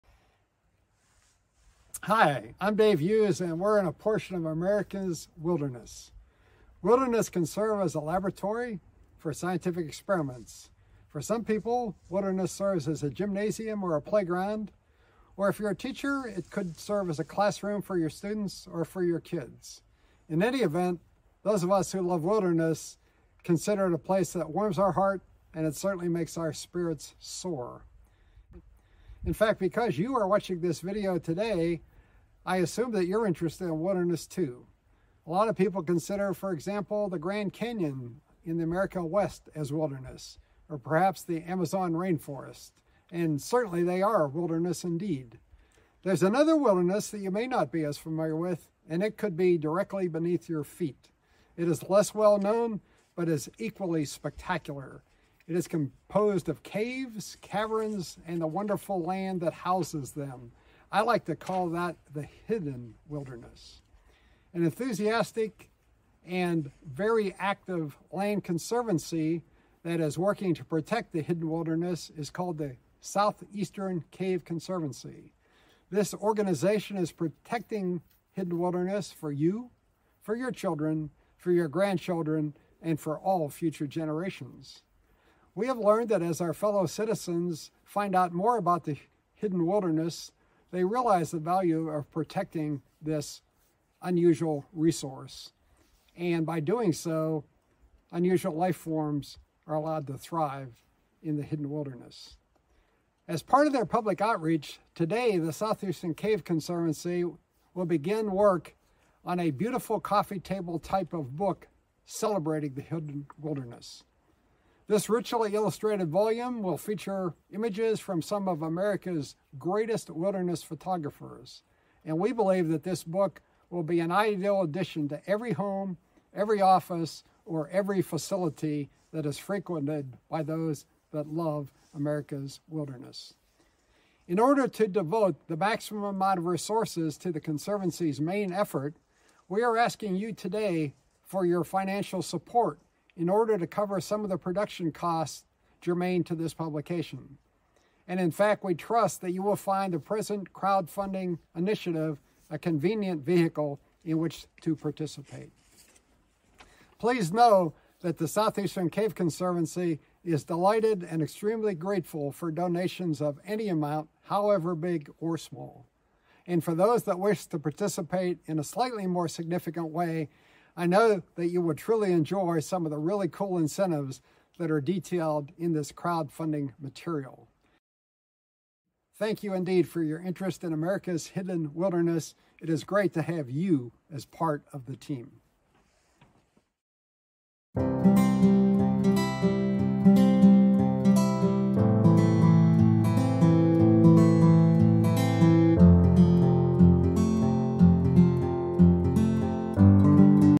The Hidden Wilderness Campaign
We are excited to announce our new campaign to raise funds for the printing of a coffee table type book titled The Hidden Wilderness. A 160-page full color book will  feature many of the Southeastern Cave Conservancy's caves.